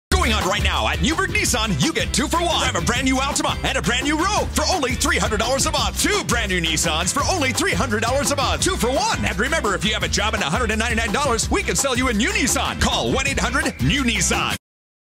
Two for One at Newburgh Nissan
Lease the 2014 Nissan Altima 2.5 S and 2014 Nissan Rogue S together starting at $300 a month! Please see the dealership for complete details.